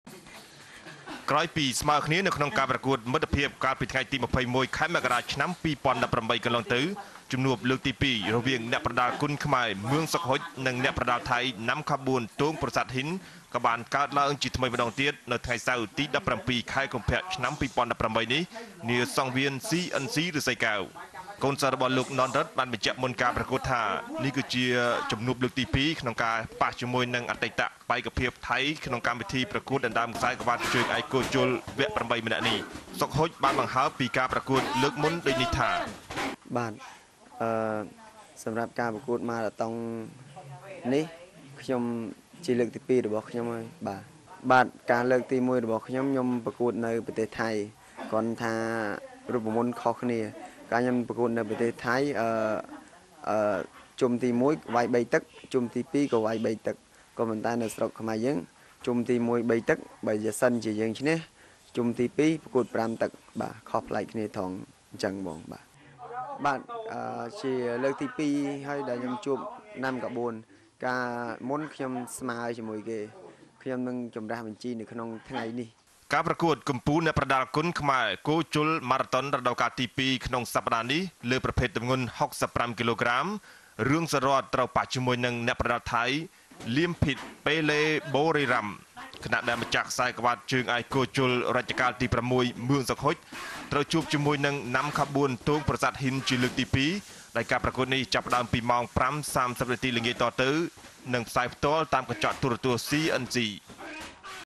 សុខហ៊ុច និយាយពីជំនួបលើកទីពីរជាមួយ ណាំខៈបួន
ន សុខហ៊ុច នឹងអ្នកប្រដាល់ថៃ ណាំខៈបួន តូងប្រាសាទហ៊ីន ប៉ះគ្នាជាថ្មីម្តងទៀតនៅថ្ងៃសៅរ៍ ទី១៧ កុម្ភៈ ២០១៨ នាសង្វៀន ស៊ី អិន ស៊ី